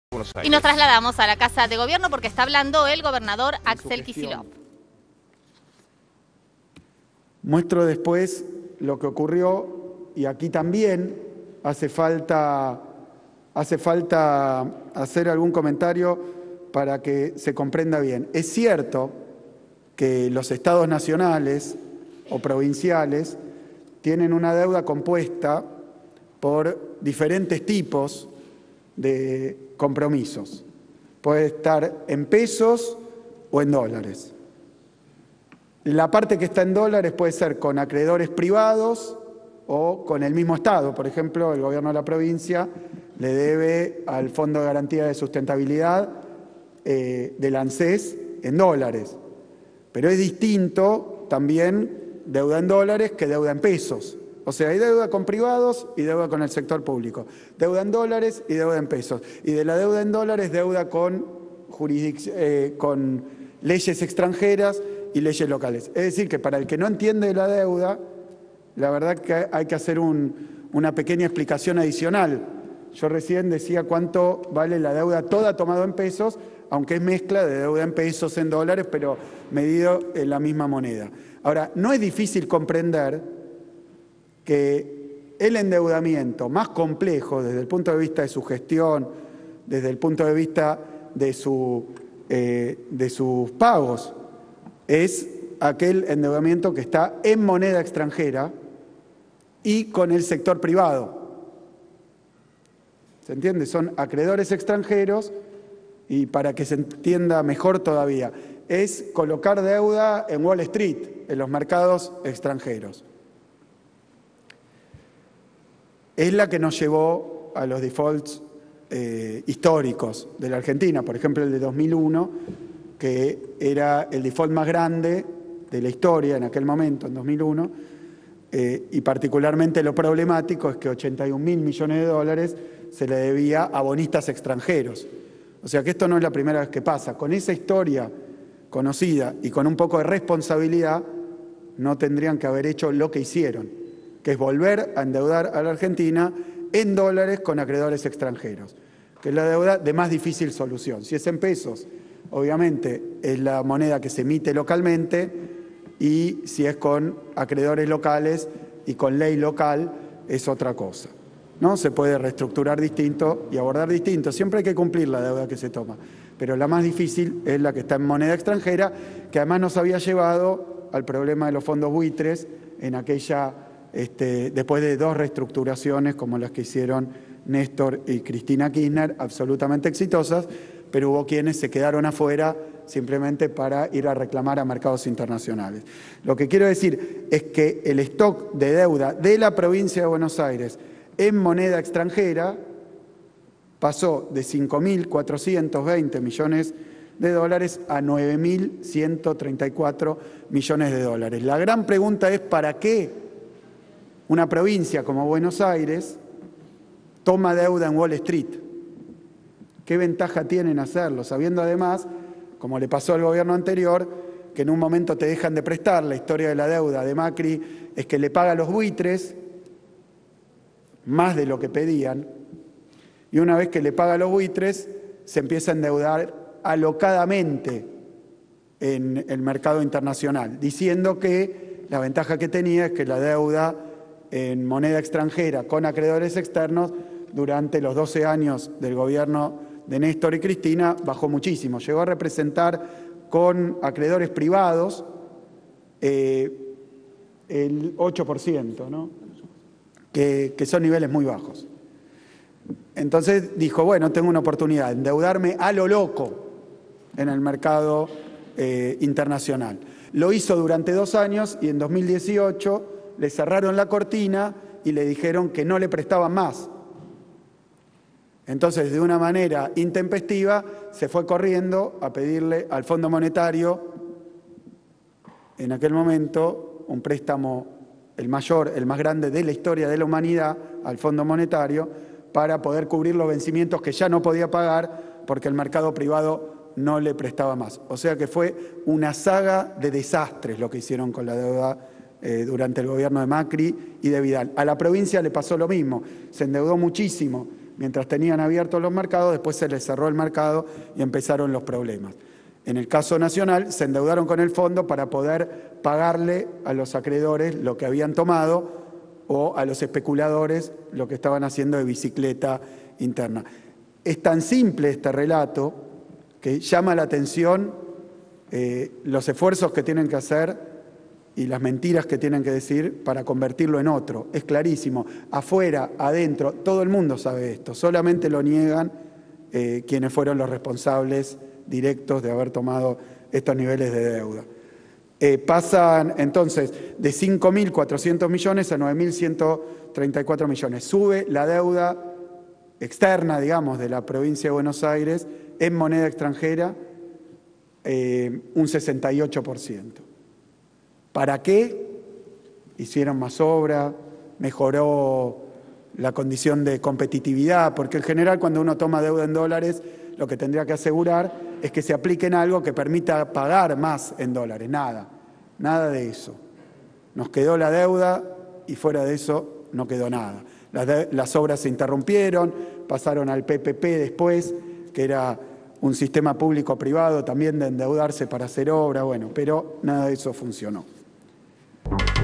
Anunciaron la reestructuración del 98% de la deuda externa bonaerense
En una conferencia de prensa encabezada por el gobernador Axel Kicillof, el ministro de Hacienda y Finanzas de la Provincia de Buenos Aires, Pablo López, anunció hoy que “hemos obtenido una adhesión suficiente para canjear prácticamente el 98% de la deuda en moneda extranjera y bajo ley extranjera”, tras concluir el viernes pasado el proceso de recepción de ofertas a la propuesta de reestructuración de esos compromisos. El Gobernador sostuvo que "siempre hay que cumplir con la deuda que se toma, pero la más difícil es la que se toma en moneda extranjera".